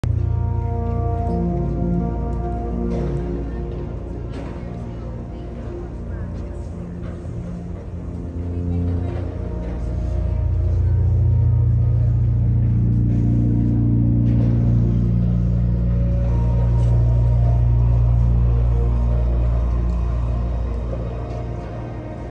Sonic Sphere, Steve Reich set (1 of ), The Shed, Hudson Yards, NYC, Friday, July 14, 2023.
Sonic Sphere, Steve Reich set, The Shed, Hudson Yards, Manhattan, NYC, 5:30pm, Friday, July 14, 2023.